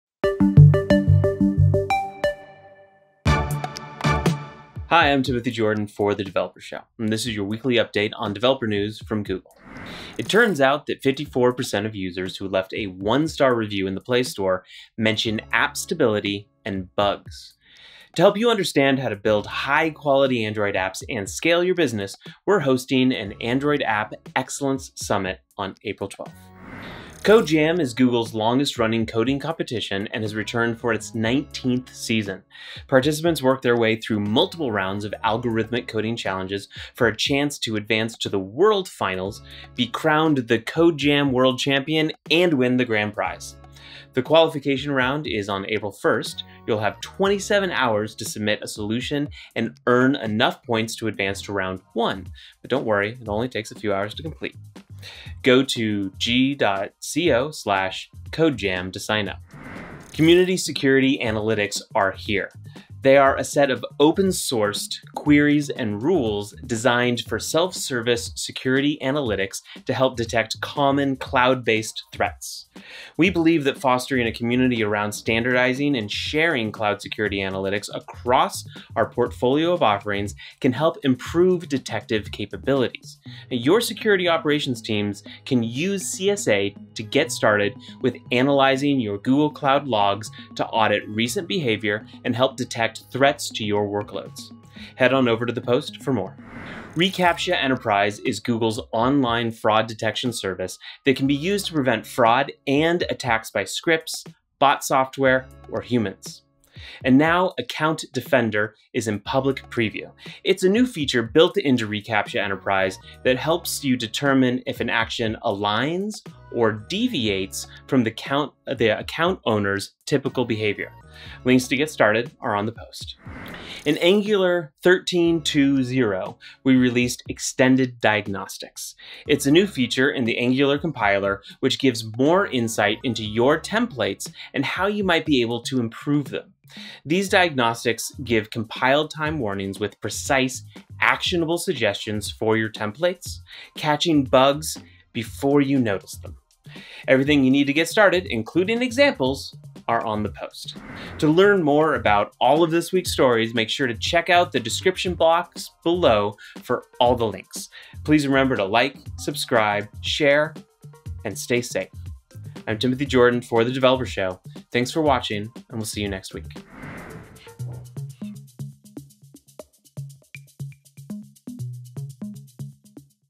Code Jam Coding Competition, App Excellence Summit 2022, and more dev news
0:00 - Introduction
0:10 - App Excellence Summit 2022
0:26 - Code Jam Coding Competition
1:00 - Introducing Community Security Analytics
1:37 - Protect your users’ accounts with reCAPTCHA Enterprise’s account defender
2:03 - Angular Extended Diagnostics
2:31 - Please remember to like, subscribe, and share.

Here to bring you the latest developer news from across Google is Timothy Jordan from Developer Relations. Tune in every week for a new episode, and let us know what you think of the latest announcements in the comments below.